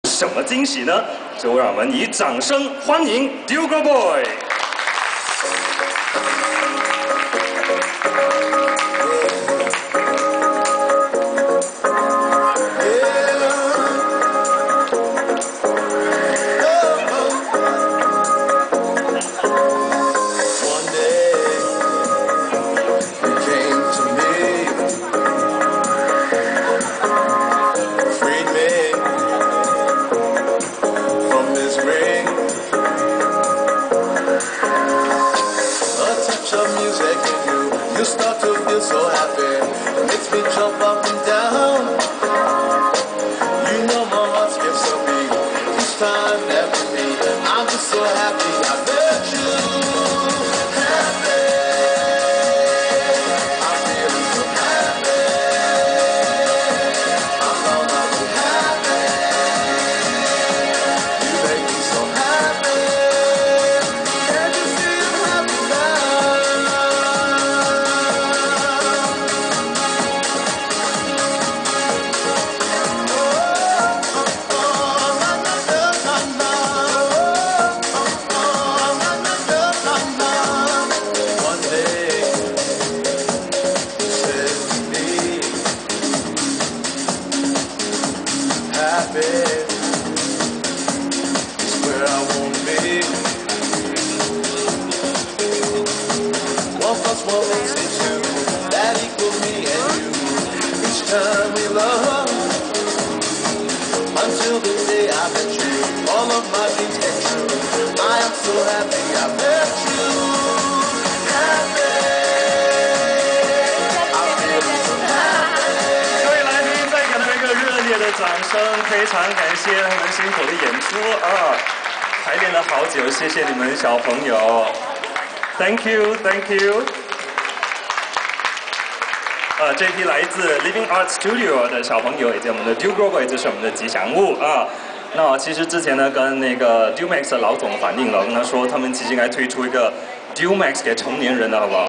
Living Arts Dance Studio
Kids Performance for Dumex Dugro New Product Launch @ One Utama